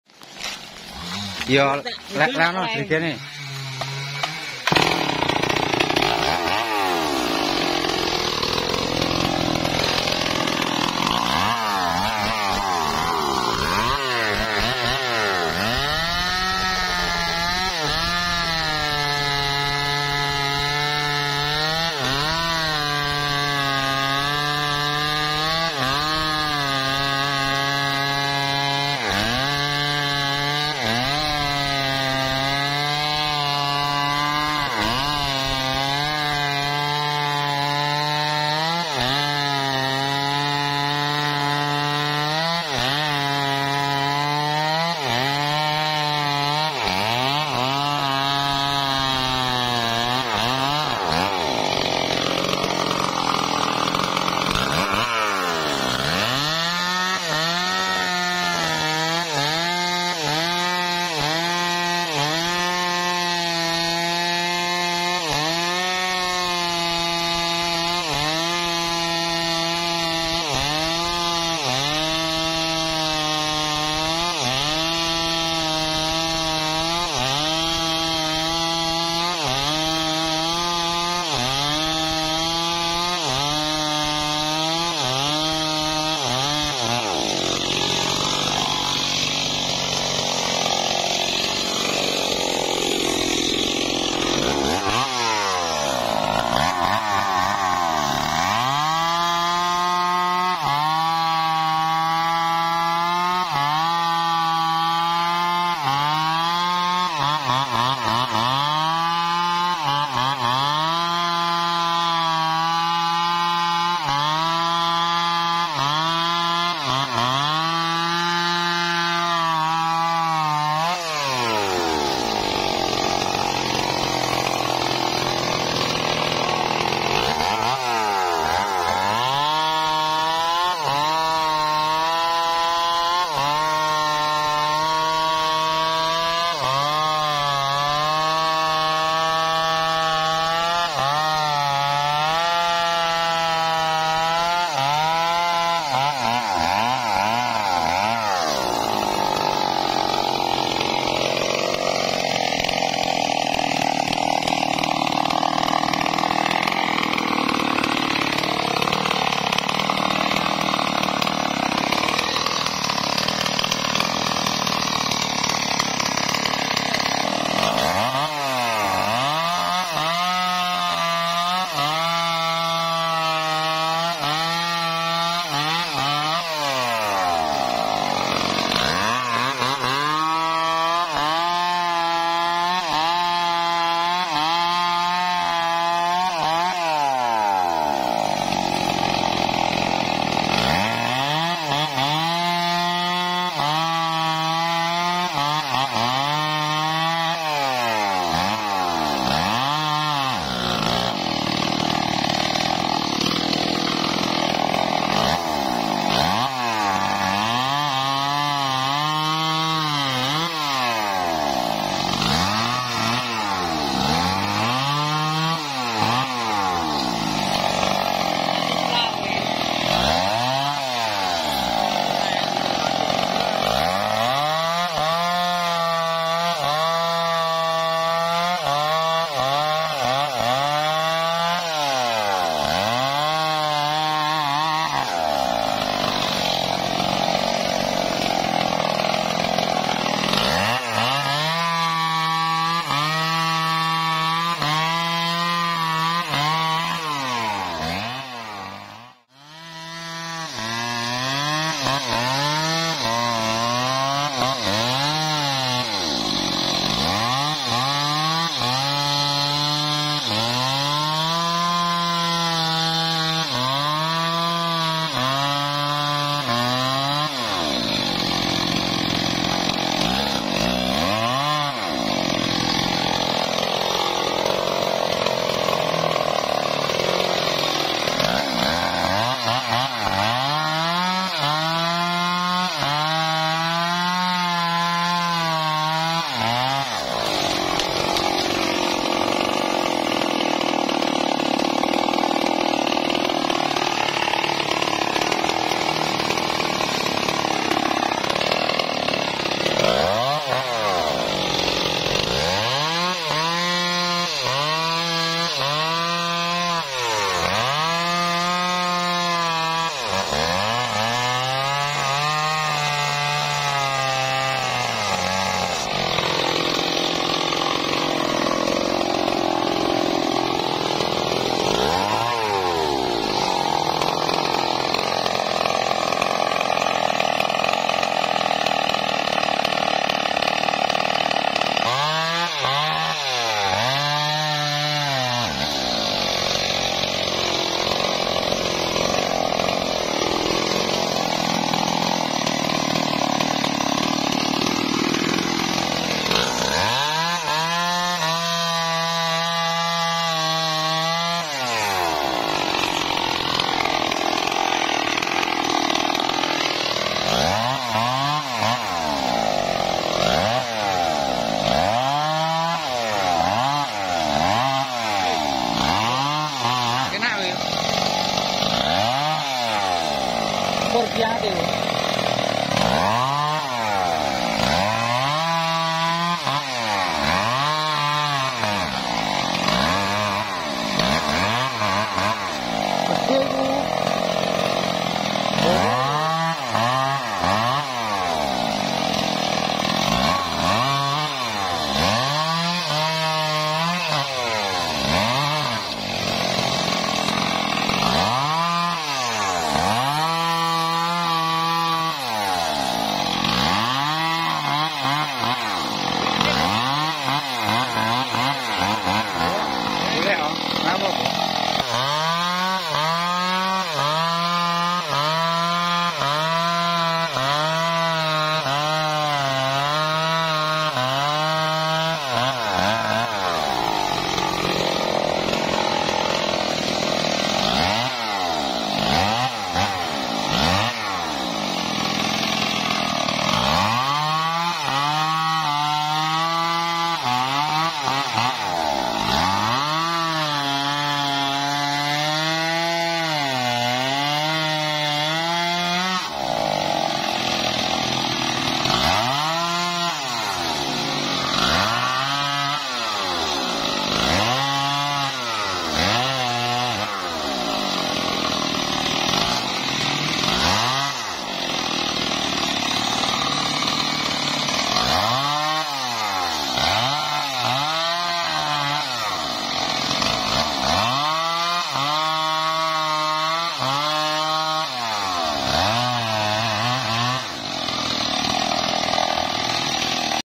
Ngebut !! Tebang pohon mahoni dan jati bahan mebel - gergaji mesin stihl
Borongan buat bahan mebel !! Kayu mahoni Indonesia - gergaji mesin stihl ms 382

Nyangkut !! menebang pohon mahoni - Gergaji mesin stihl ms 382,
tebang pohon menggunakan gergaji mesin stihl ms 382 oleh operator senso kayu yang sudah profesional, kayu ini nanti akan langsung dimuat ke truck dan dibawa kesawmill untuk dibuat usuk bahan bangunan rumah dan mebel kayu